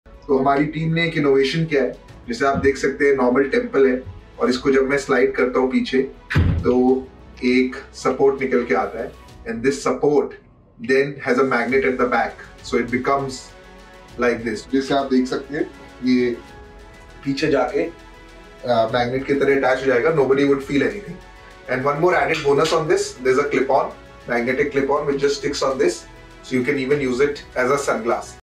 Peyush Bansal Talks About The Eyewear Innovation At Lenskart
The all time favourite magnetic clip-ons that go from eye to sun in a snap, have a cool new addition.
Sliding rubber retainers that lock around the head make sure that your eyewear stays put, always! Ultra-cool tints, classic shapes and a modern matte finish gives this collection the perfect balance of fashion and function.
Get ready to elevate your everyday look, indoors and outdoors with Lenskart Air Switch Grip.

Swap it. Lock it. Rock it.